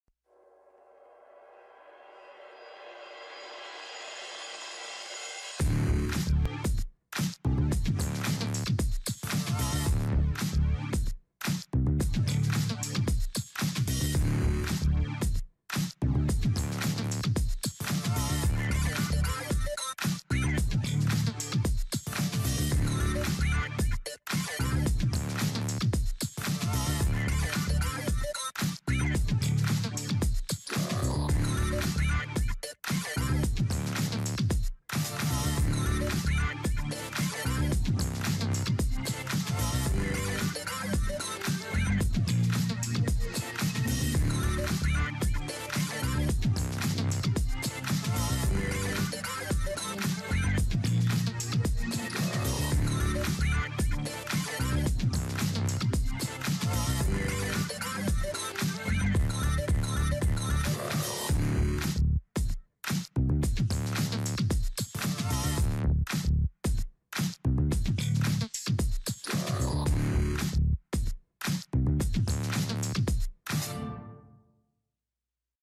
Mais häckseln bei Agrarservice Baumann made by DaKappO
Mal nen kleinen Ausschnitt vom häckseln beim Agrarservice Baumann. Mal als Abfahrer mal als Häckslerfahrer.